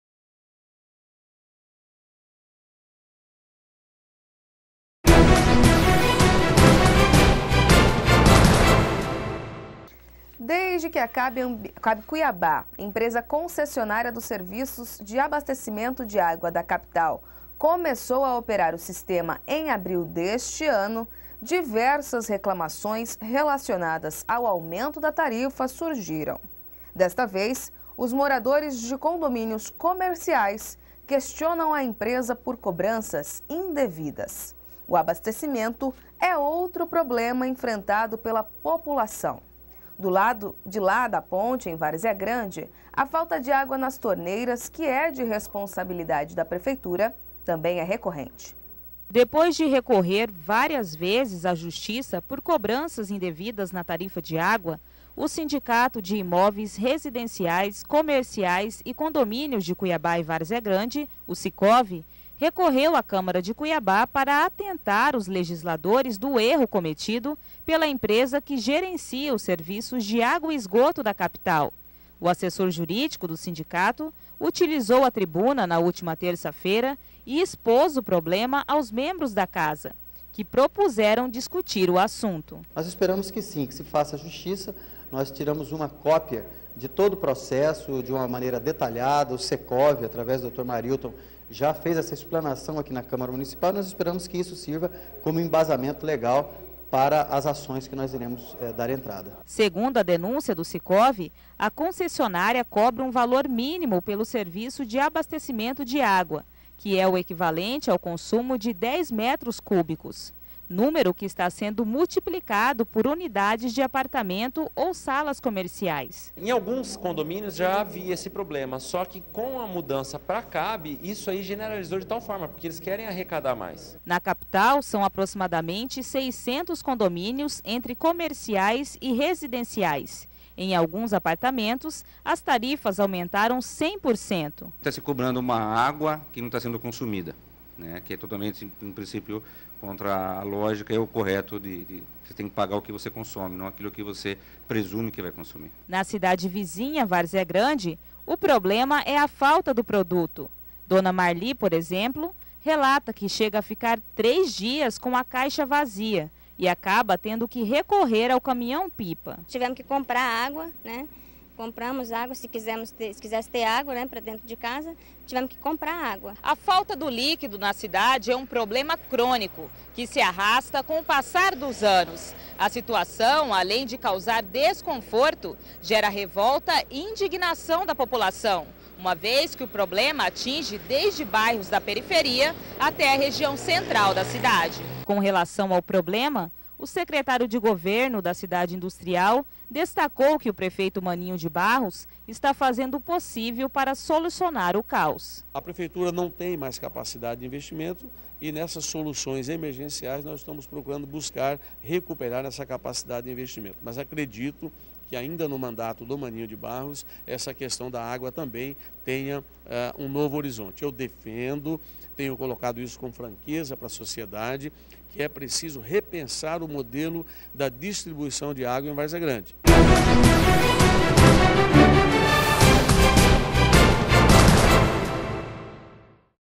População reclama de problemas no abastecimento de água
Desde que a CAB Cuiabá, empresa concessionária dos serviços de abastecimento de água da Capital, começou a operar o sistema em abril deste ano,  diversas reclamações relacionadas ao aumento da tarifa surgiram. Desta vez, os moradores de condomínios comerciais questionam a empresa por cobranças indevidas. O abastecimento é outro problema enfrentado pela população. Do outro lado da ponte, em Várzea Grande, a falta de água nas torneias, que é de responsabilidade da prefeitura, é recorrente.